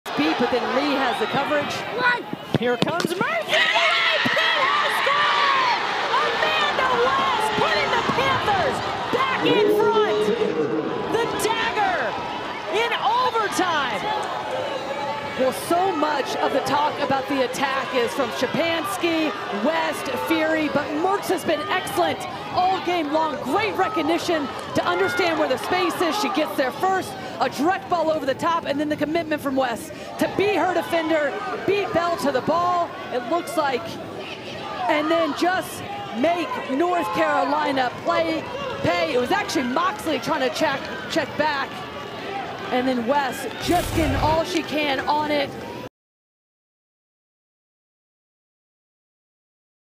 Pitt's Amanda West Gets The Go-Ahead Goal In Overtime
In the first overtime period, Pitt was able to go on a counterattack and Landy Mertz passed the ball to the front of the net where it found Amanda West for the header that she was able to put into the back of the net. The goal put Pitt in front with a second extra period still to play.